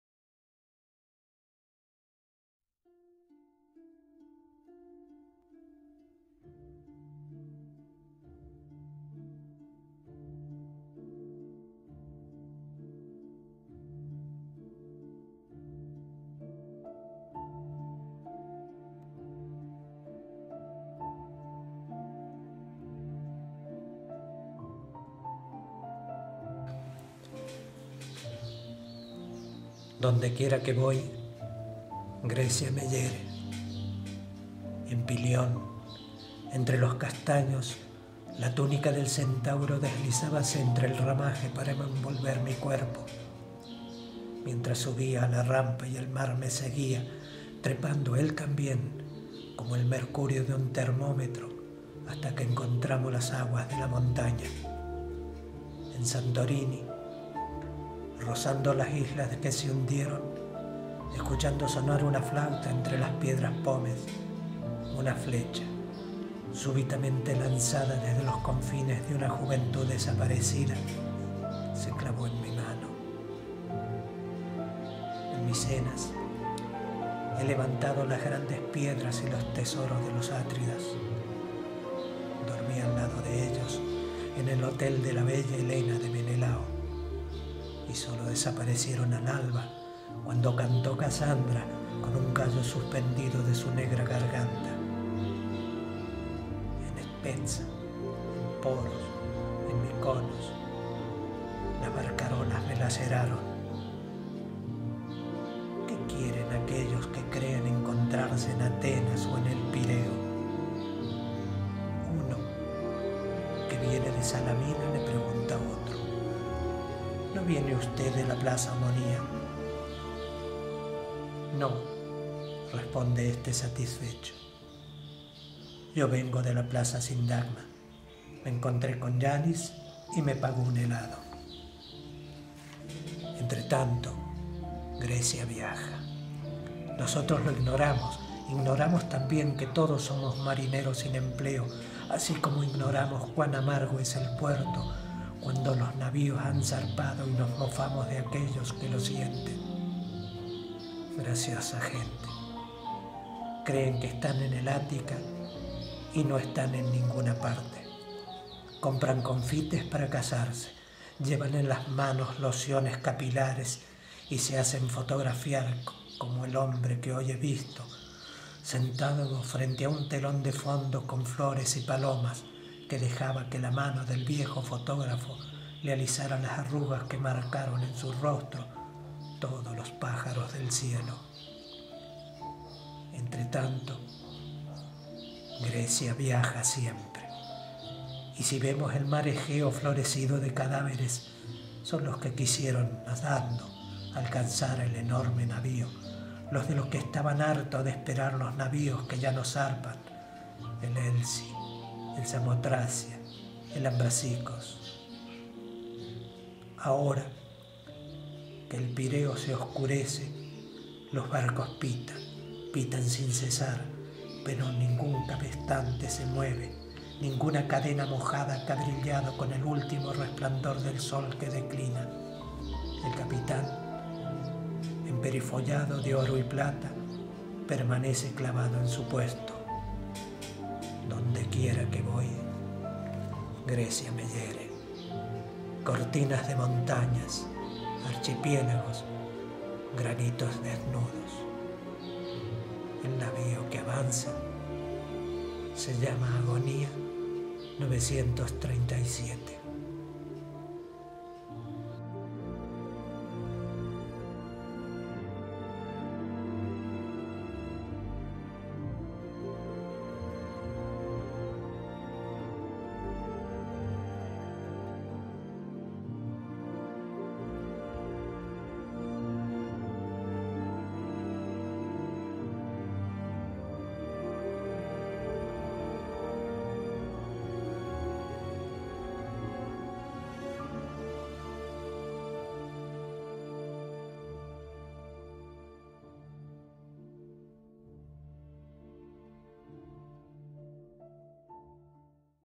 DONDEQUIERA QUE VOY, GRECIA ME HIERE [GIORGOS SEFERIS]
Poema de Giorgos Seferis perteneciente al libro El zorzal y otros poemas", escrito a bordo del "Aulis", aguardando el instante de levantar las velas, en el verano de 1936, y leído por Antonio Tello.